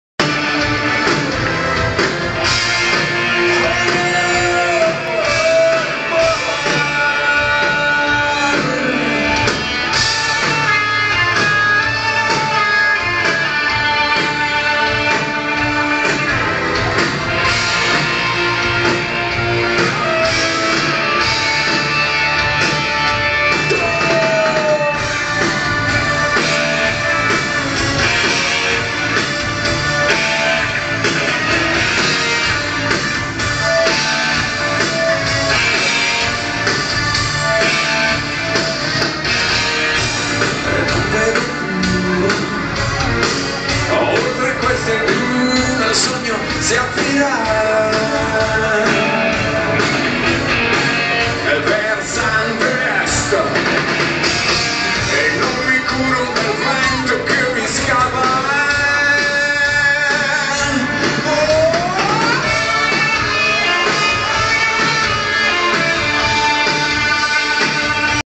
Litfiba a Sogliano al Rubicone
I Litfiba in concerto a Sogliano al Rubicone (Emilia Romagna) il 30 Giugno 2013